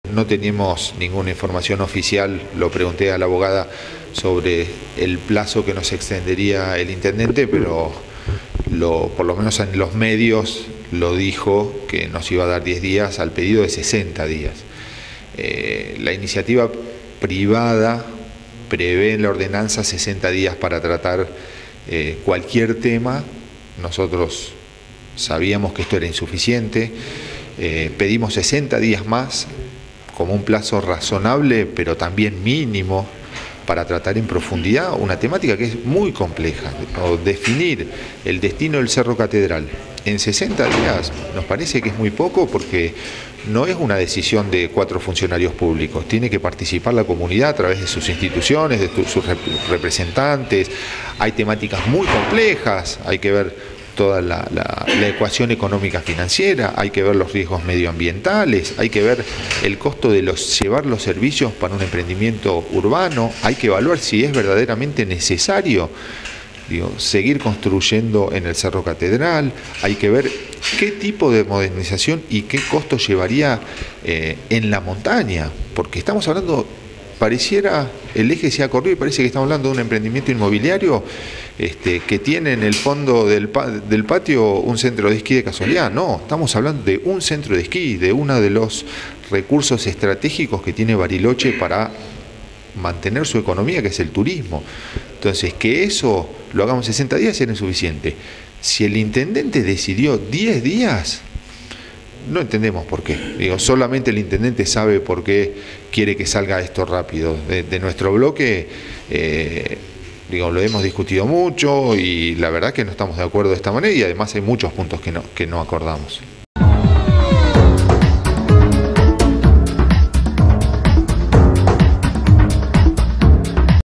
CONCESIÓN DEL CATEDRAL: CONCEJALES DEL FPV RECLAMAN 60 DÍAS MÁS DE PLAZO PARA DEBATIR
Concejales del Frente para la Victoria piden al ejecutivo más tiempo para poder debatir el contrato de concesión del Cerro Catedral.